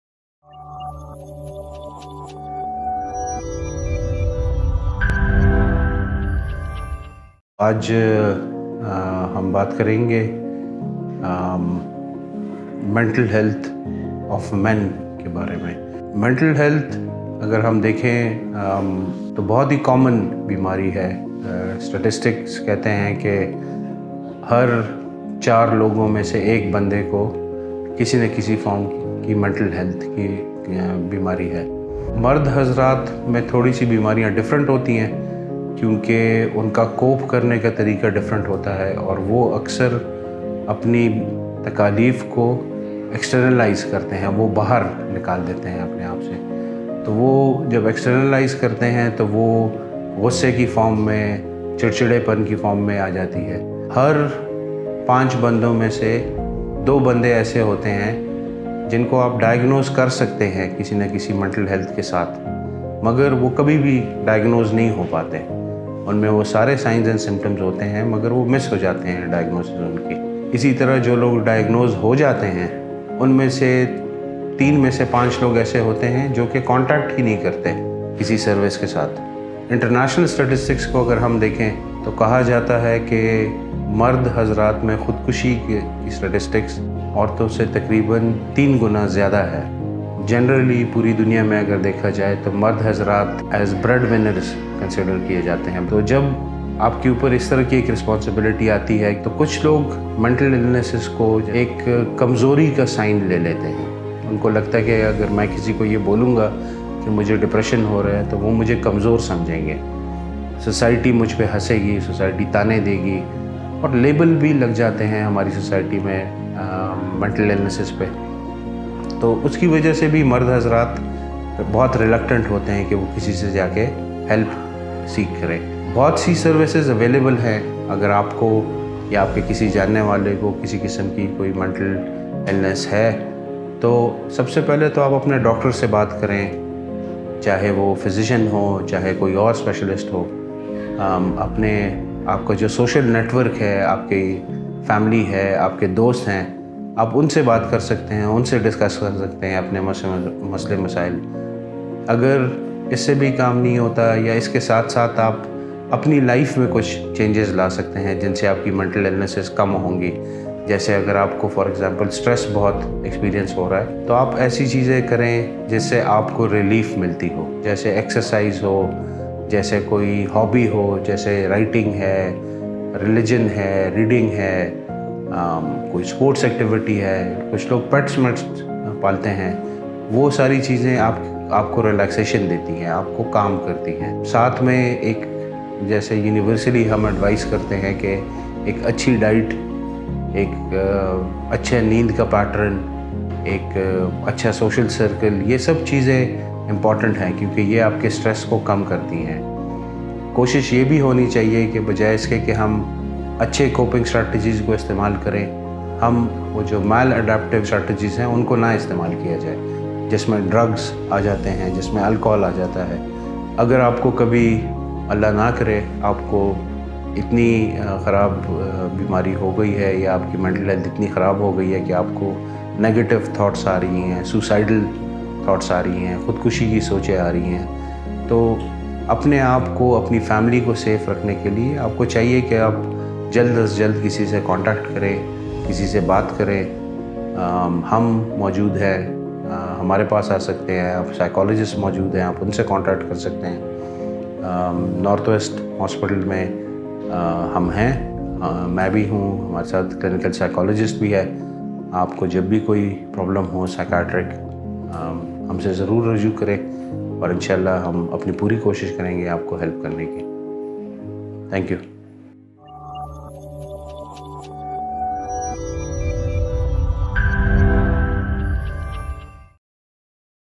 International Men's Health Awareness Week - Dr. Muhammad Noman Wazir
International Men's Health Week is observed to recognise the global issues, contributions, and achievements of men and boys in the nation, union, society, community, family, marriage, and childcare.

Dr. Muhammad Noman Wazir (Consultant Psychiatrist at Northwest General Hospital) highlights issues related to men and the stigmas our society has associated with men's mental health. Every 2 in 5 men can be diagnosed with some type of mental problem. Those left either do not think they have any issues or have never contacted any service. The probability of suicide is higher in men than in women. It is primarily the outcome of prevalent stereotypes associated with mental illnesses that make people reluctant to seek professional help. If left unaddressed, these mental illnesses and problems are often the cause of drug abuse. One should choose to recover from mental illnesses through productive exercises and habits.